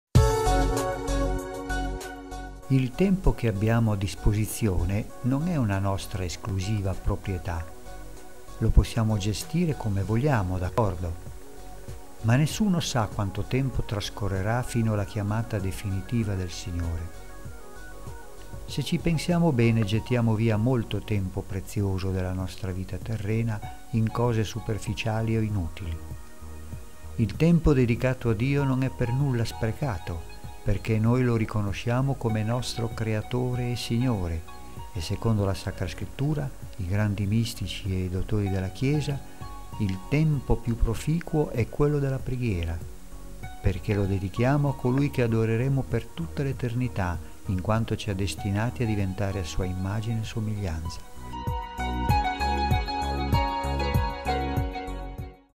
IL TEMPO DEDICATO A DIO NON È PERSO
Il tempo che abbiamo non è una nostra esclusiva proprietà. Lo possiamo gestire come vogliamo, d’accordo. Ma nessuno sa quanto tempo trascorrerà fino alla chiamata definitiva del Signore.
Ma se ci pensiamo bene gettiamo via molto tempo prezioso della nostra vita terrena in cose superficiali o inutili.
Il tempo dedicato a Dio non è per nulla sprecato perché noi lo riconosciamo come nostro Creatore e Signore e, secondo la Sacra Scrittura, i grandi mistici e dottori della Chiesa, il tempo più proficuo è quello della preghiera, perché lo dedichiamo a Colui che adoreremo per tutta l’eternità in quanto ci ha destinati a diventare a sua immagine e somiglianza.

CONCETTA BERTOLI - La donna che vide la terza guerra mondiale